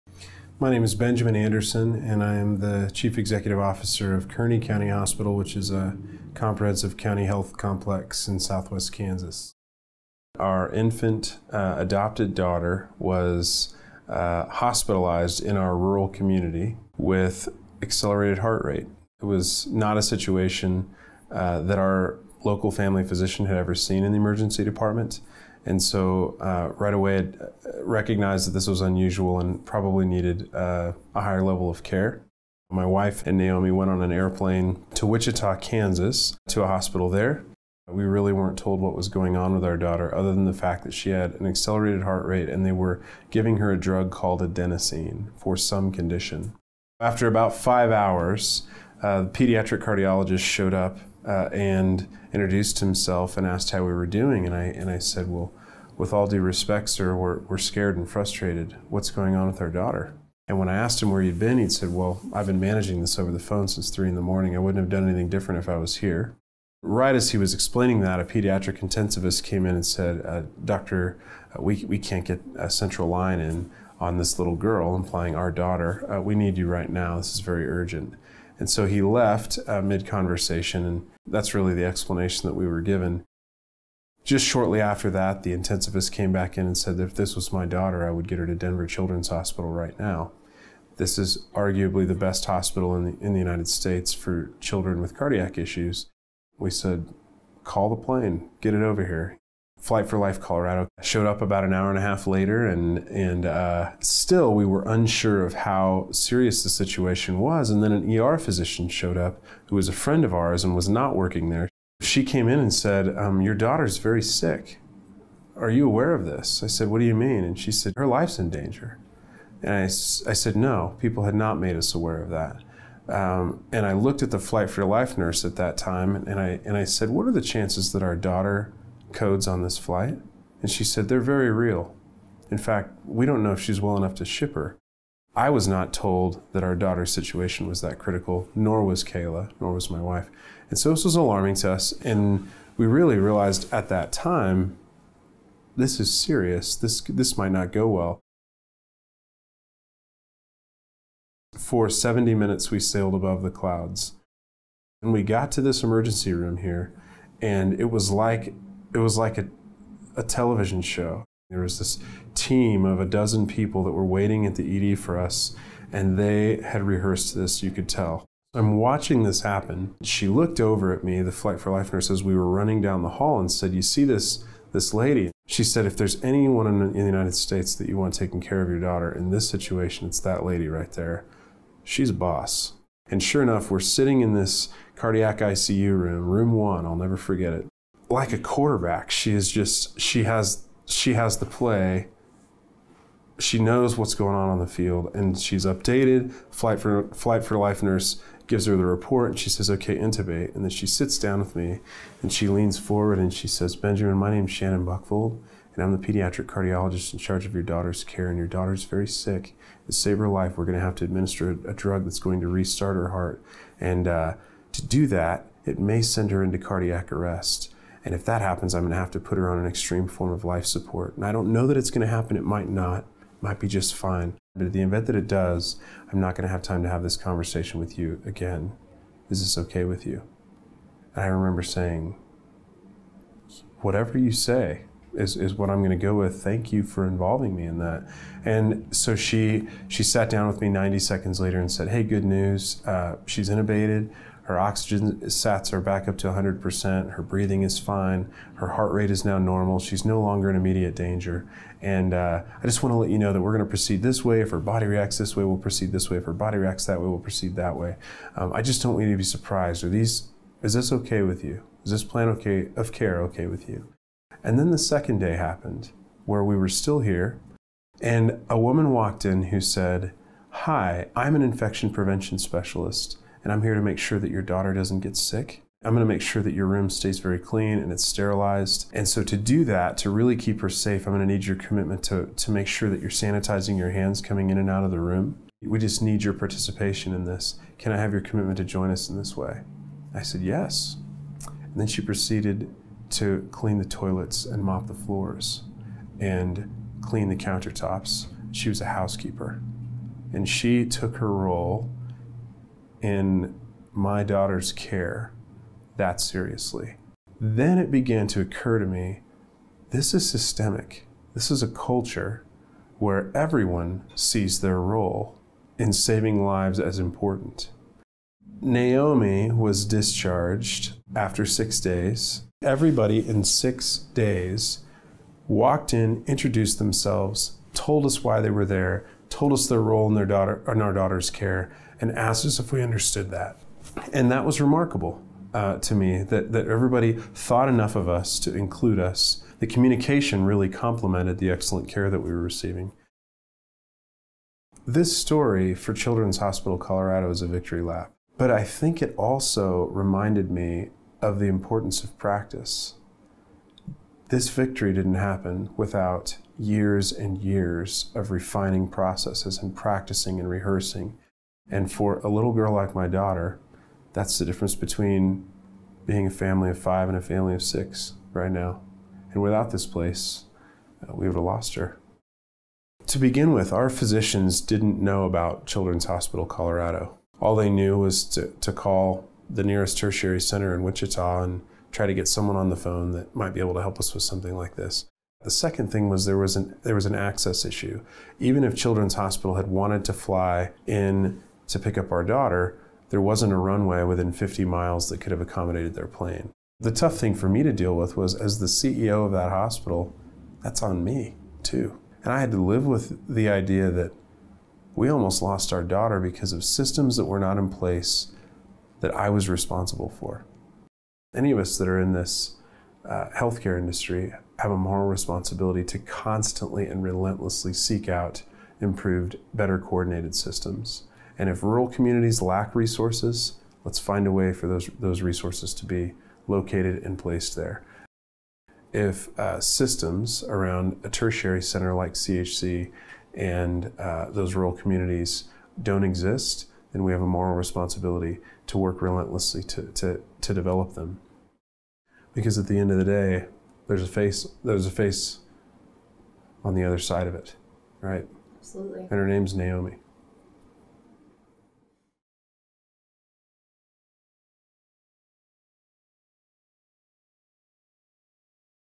Three hospitals, two states, 24 hours: how Children's Colorado saved Benjamin's daughter
Hear the compelling story of Benjamin Anderson, CEO of Kearny County Hospital in Lakin, Kansas, who found himself at Children's Hospital Colorado when his daughter became sick and needed expert care very quickly.